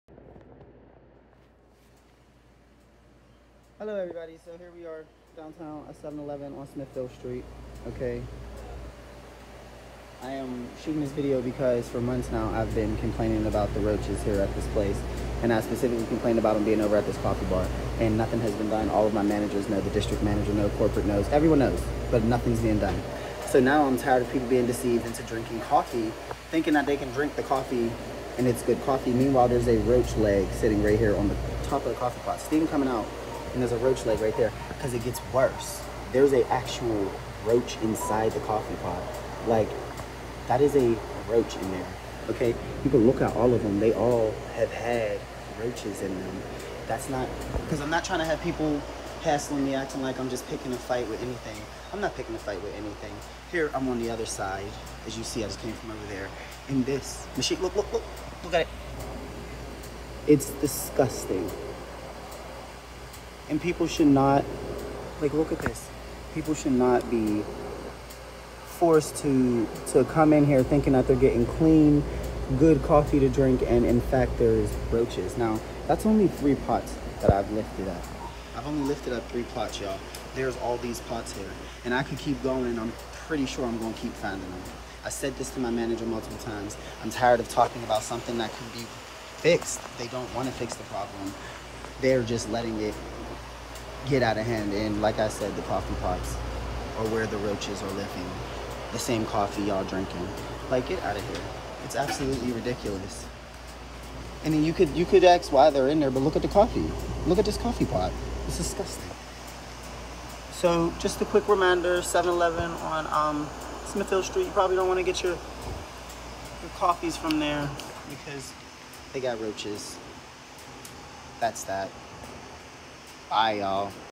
Convenience Store Coffee Pots Infested with Roaches || ViralHog
Occurred on January 5, 2020 / Pittsburgh, Pennsylvania, USA

"I’ve worked at this 7/11 for 6 months, and they have been infested with roaches the entire time. I made complaints to managers and corporate and nothing was done. I finally realized the roaches had infested the coffee pots and had enough. I shot my video and posted it. I have reached out to corporate many times and they only threatened to terminate me."